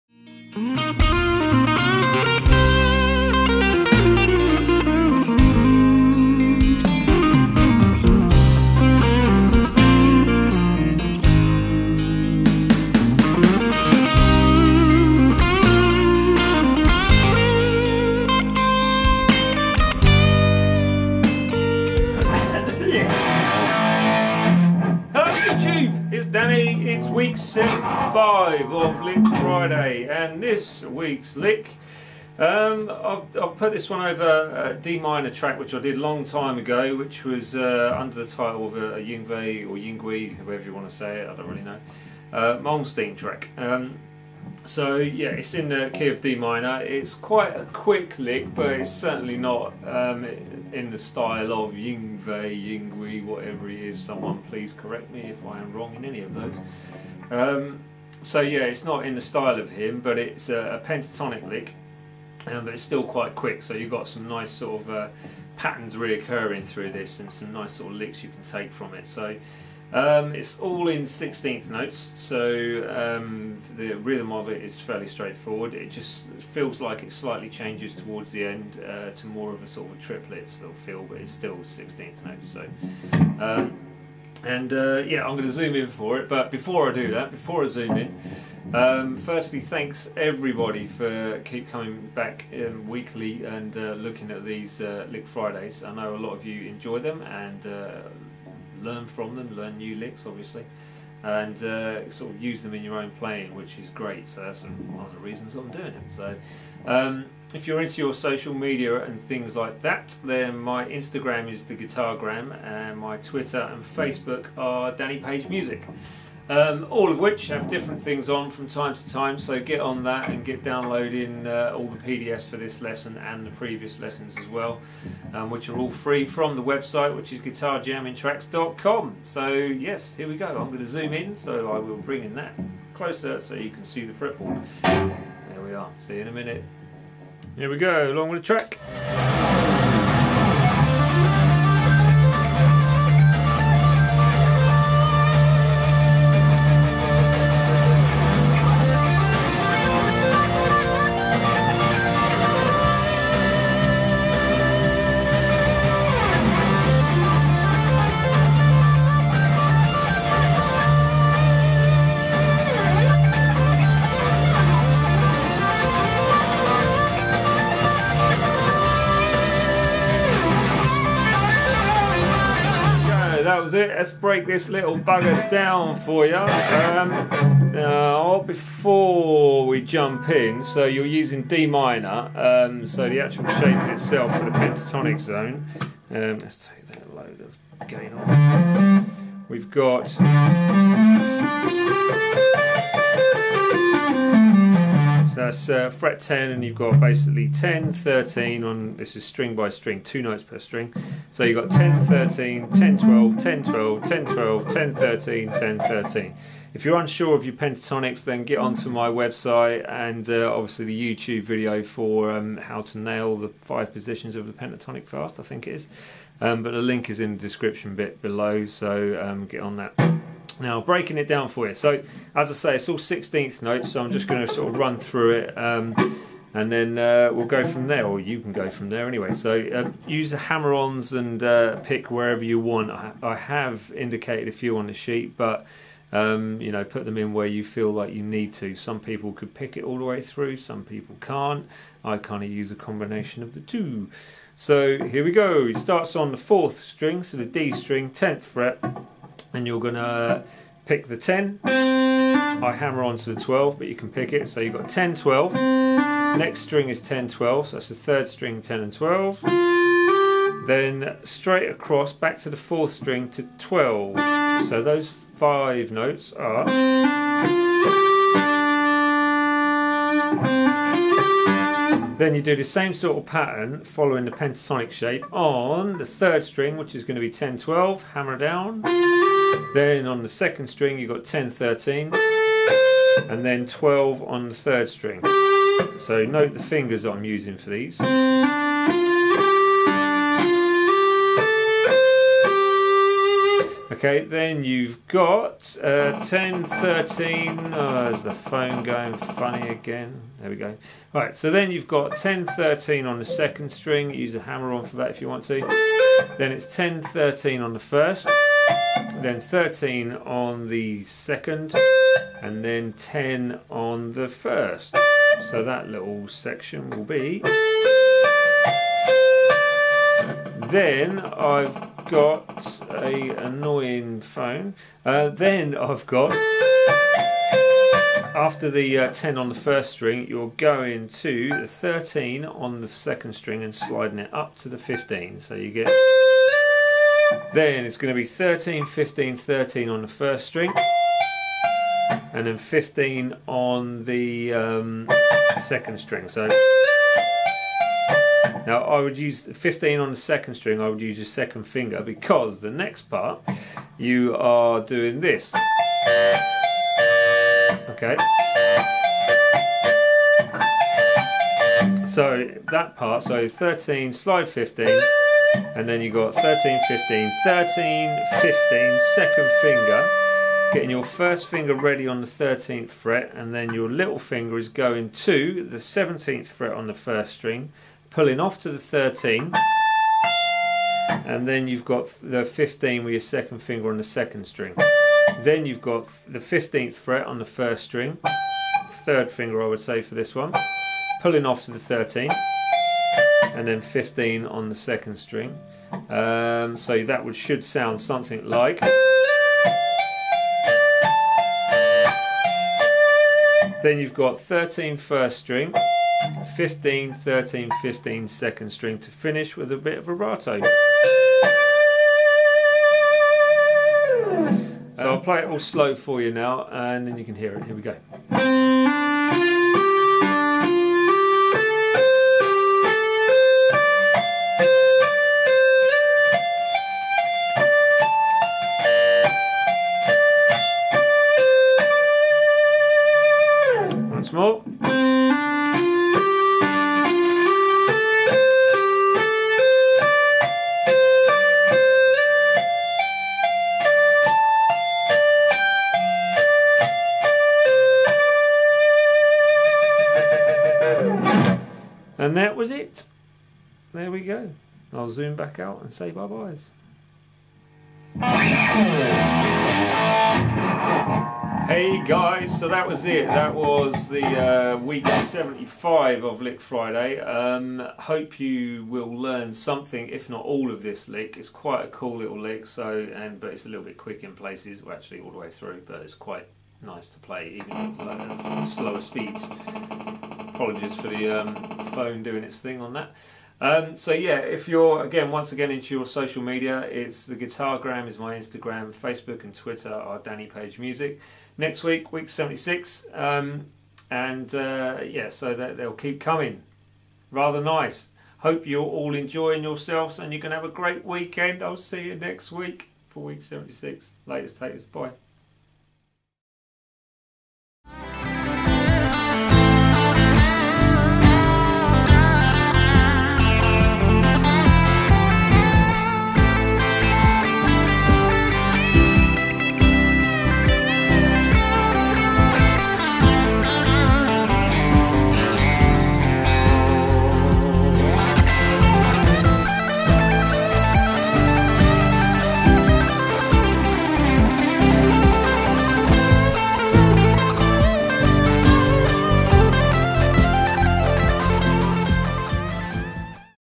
Guitar Lick Friday - Week 75 - Dm Rock 16th note lick
Week 75 (Rock 16th note Dm lick) of "Lick Friday"! Have fun with this one and I will hope to see you back here next Friday!! Cheers, Danny!

Gear used;
Lumix G6
Gibson Les Paul Studio
Marshall JVM2205c
Rode stereo VideoMic Pro